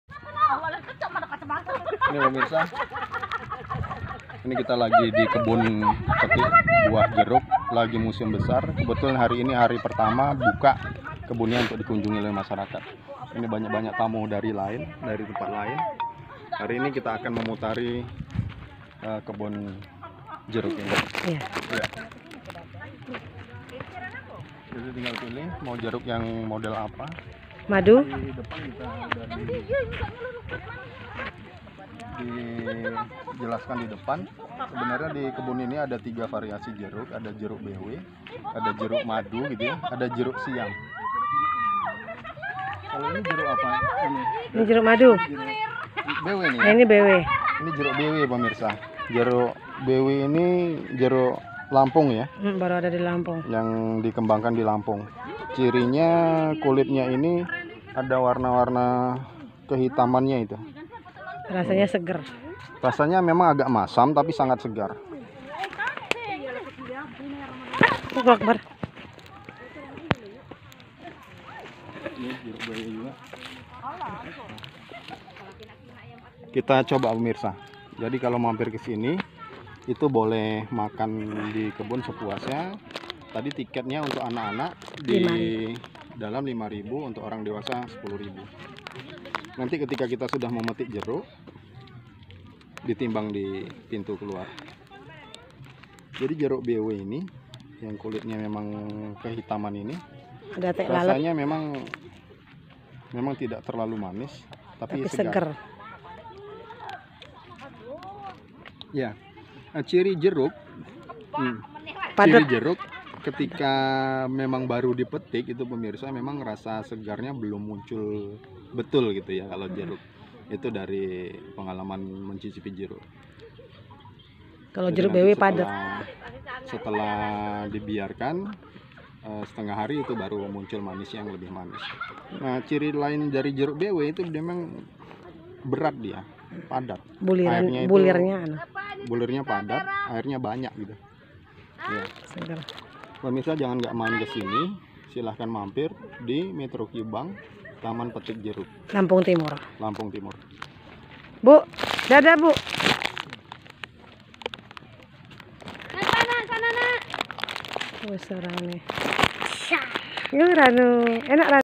Panen jeruk langsung dari pohon...
Jeruk tinggal pilih sesuai keinginan.